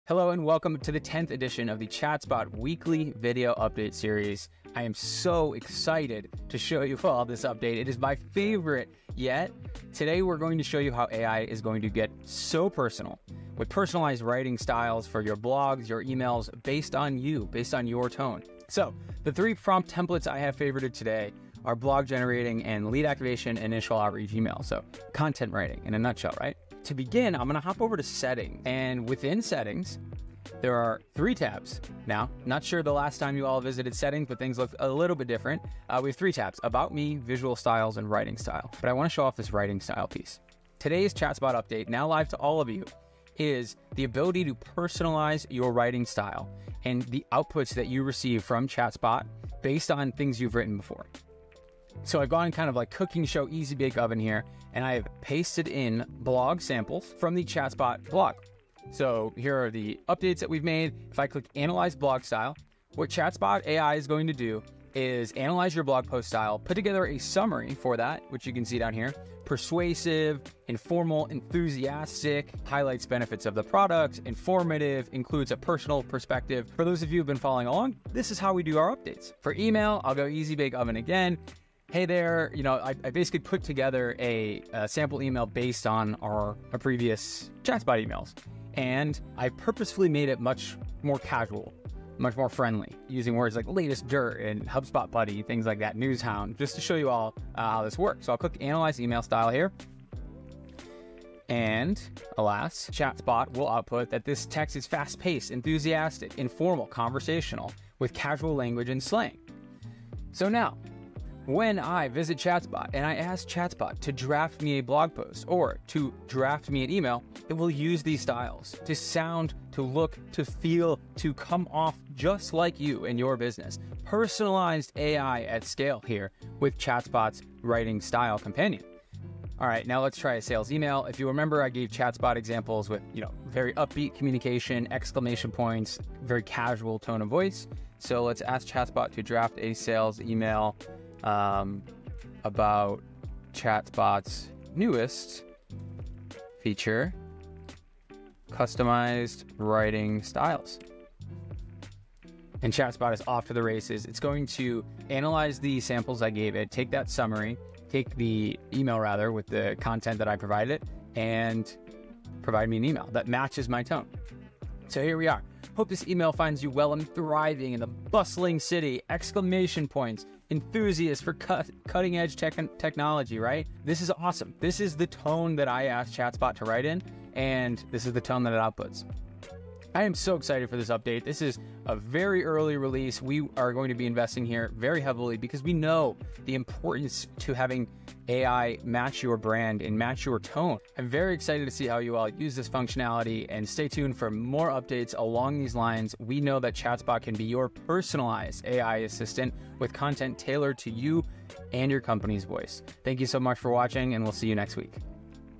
Generate on-brand blog posts and personalized email content with ChatSpot | HubSpot AI
Generic messaging yields to generic results - this is more evident than ever with generative AI creating content that lacks personalization and doesn't match your style. With this week's updates, ChatSpot is changing that by making personalization much easier - getting to know you, your brand, and your style. You no longer need to provide deeply crafted prompts every time you want to write a blog post or draft an email. Instead, ChatSpot now has two powerful settings that make ChatSpot AI your AI - understanding your style and using it to improve Blog Posts and Emails generated in the product, every time.

00:26 Have your blog post writing style analyzed
01:19 Have your email writing style analyzed
02:09 Generate a sales email using based on your writing style